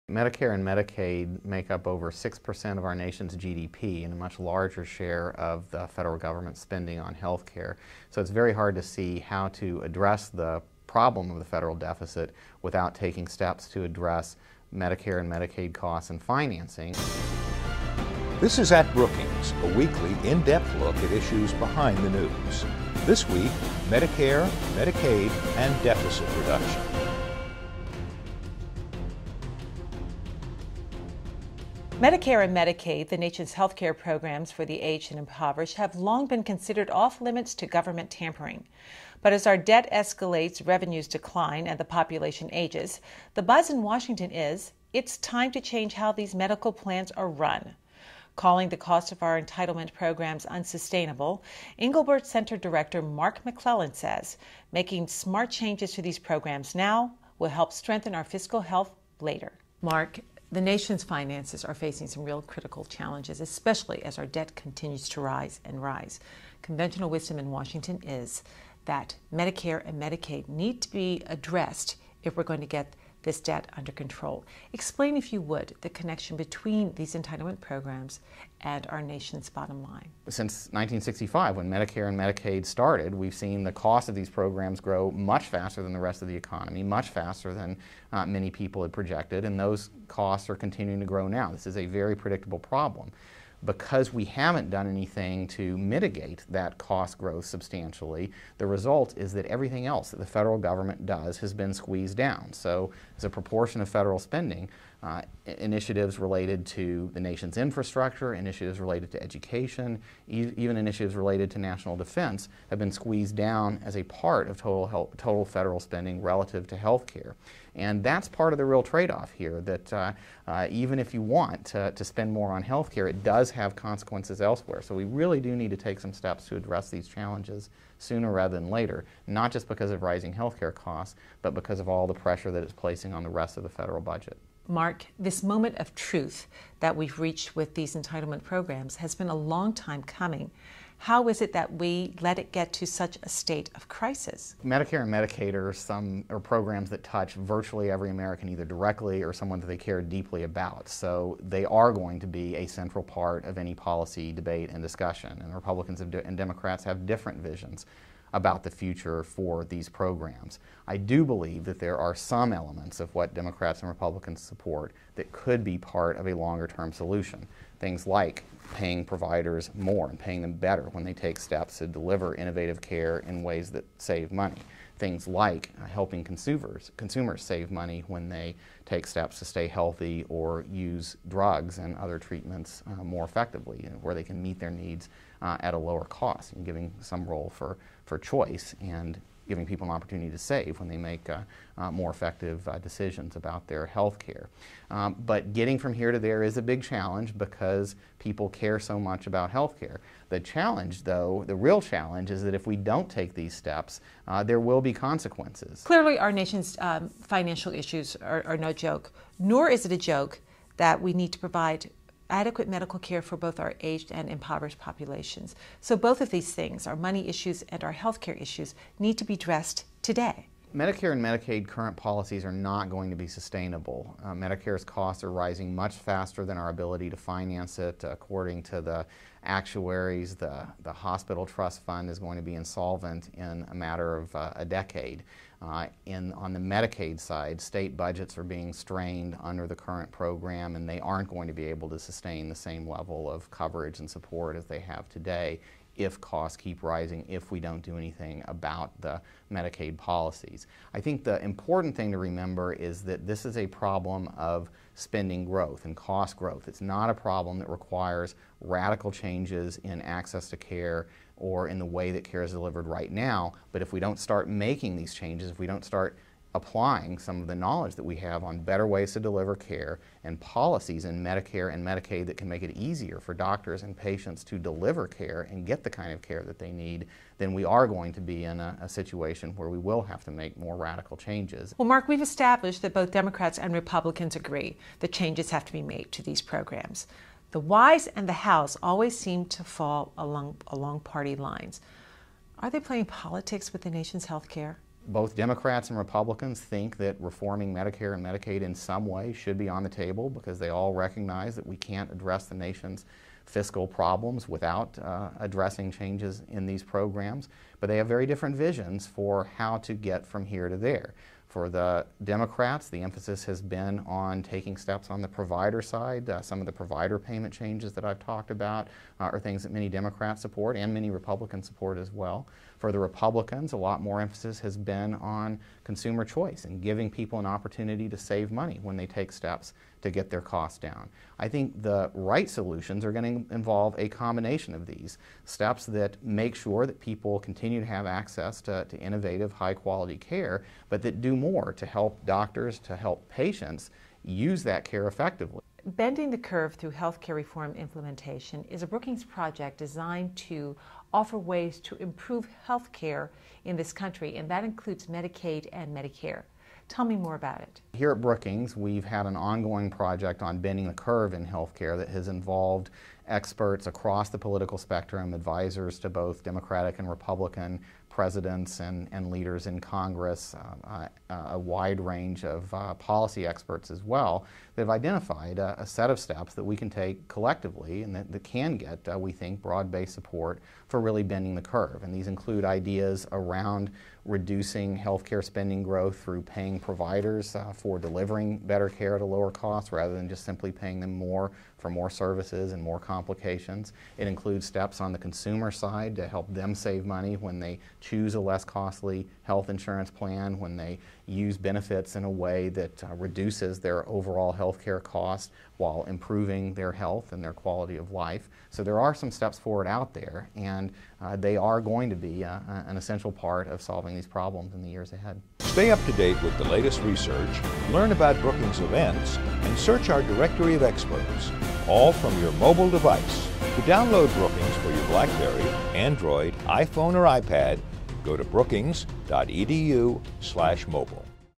Improve Medicare, Medicaid Now to Avoid Radical Changes Later
Mark McClellan: Since 1965, when Medicare and Medicaid started, we've seen the cost of these programs increase much faster than the rest of the economy, and today they're only continuing to grow. More